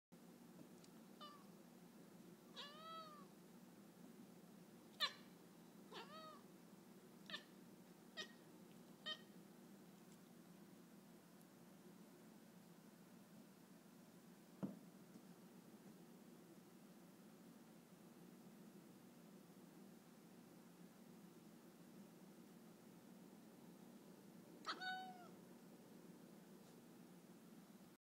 Chirping cat is chirping
Pokeweed talking noise to a bird just outside the window.